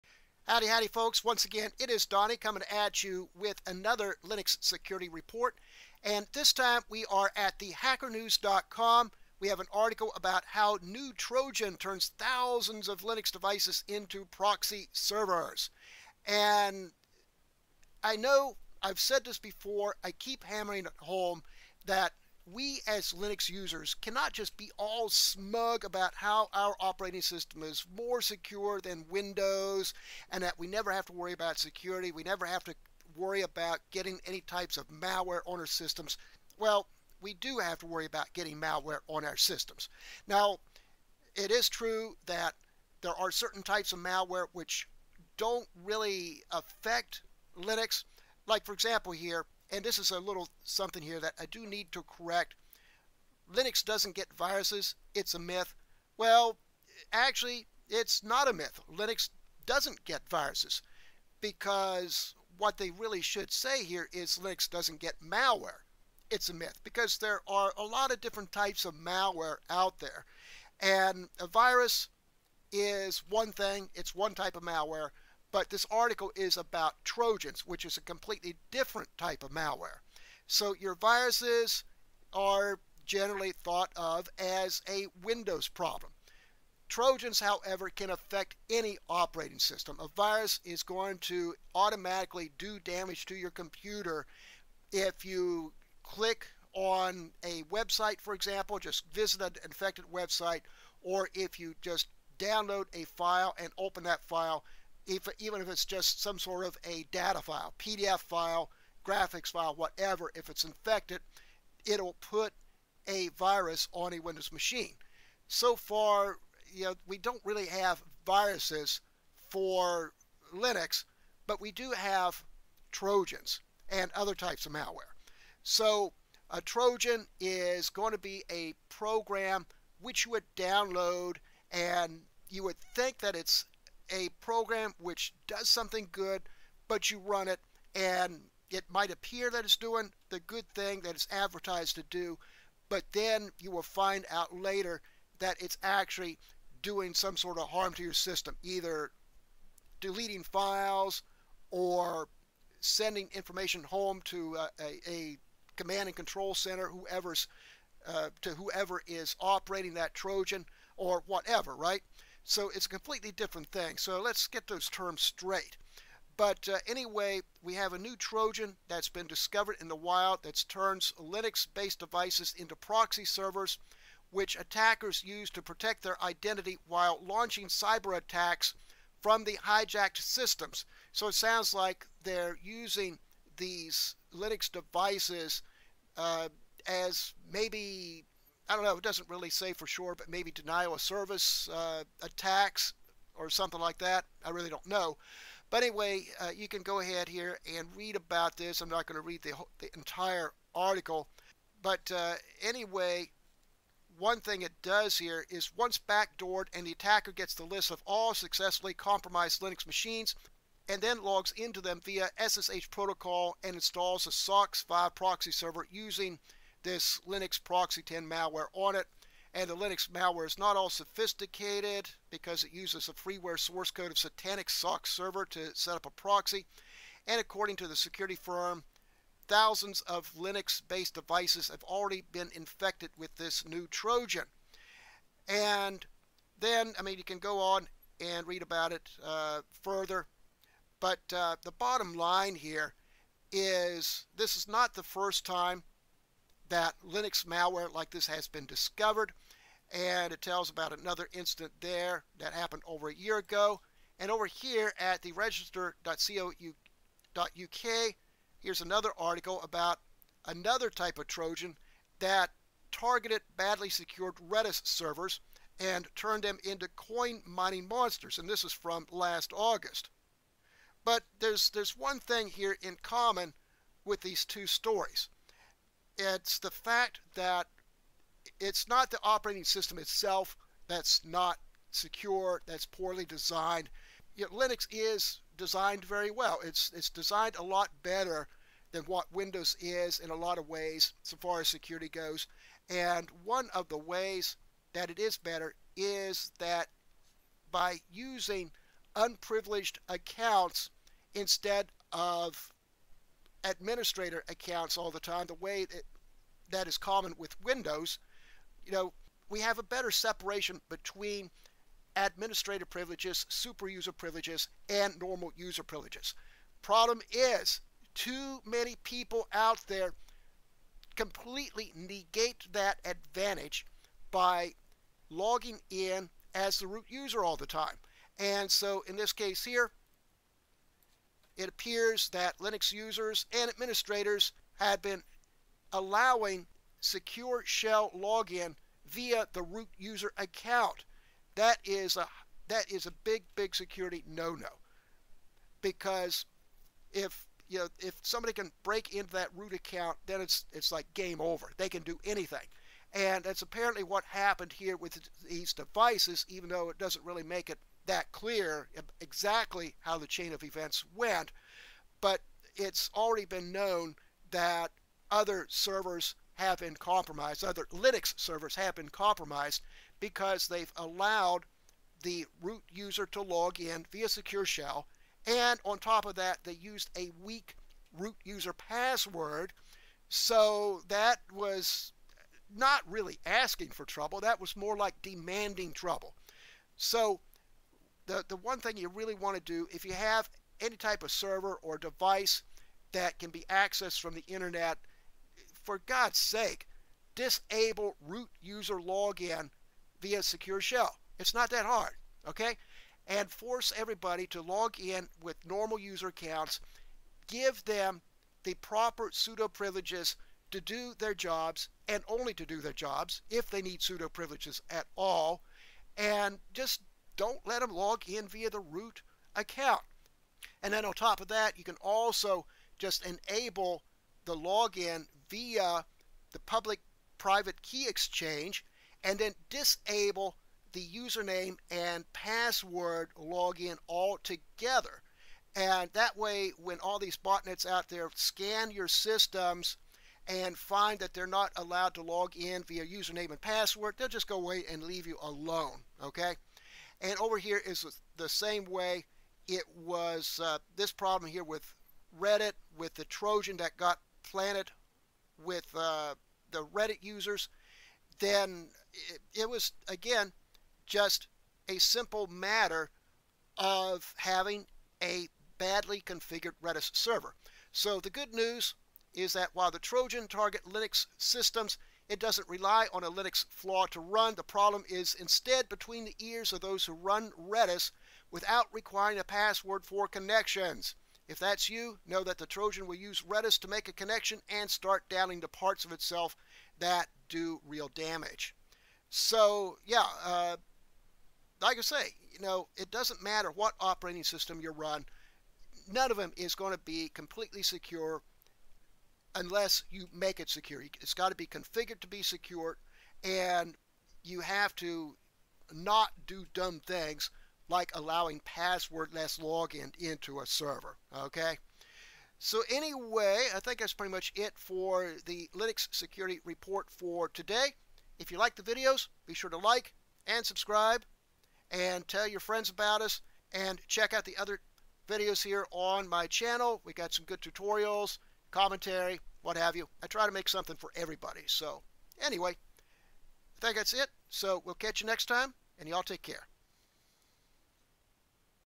New Trojan malware infects Linux Devices. Linux devices used for attacks.
A new kind of Trojan malware for Linux has been found on Linux devices.  The infected devices are then used to launch cyber attacks on other devices.  Operating system security is important, but even to have Linux security, you need to have your machines configured correctly.